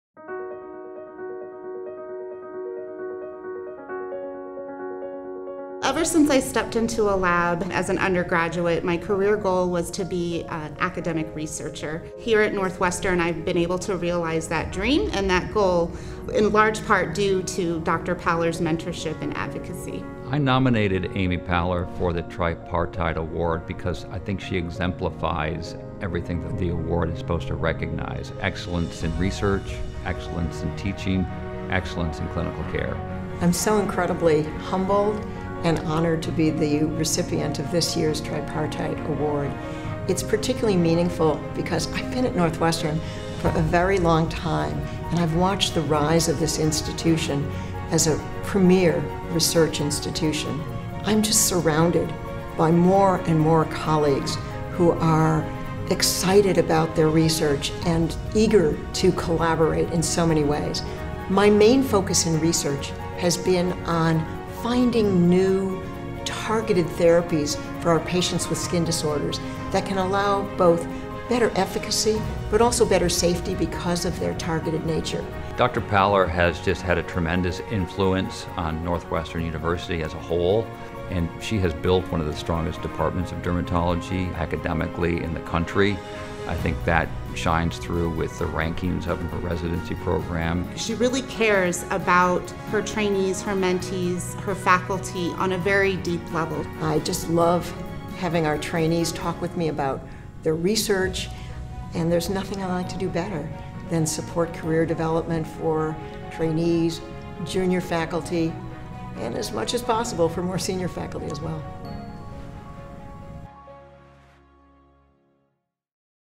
2022 Tripartite Award: Amy Paller, MD
Amy Paller, MD, has been named the winner of the 2022 Tripartite Legacy Faculty Prize in Translational Science and Education at Northwestern University Feinberg School of Medicine.

Paller is the chair of and Walter J. Hamlin Professor of Dermatology and director of the Skin Biology and Diseases Resource-Based Center.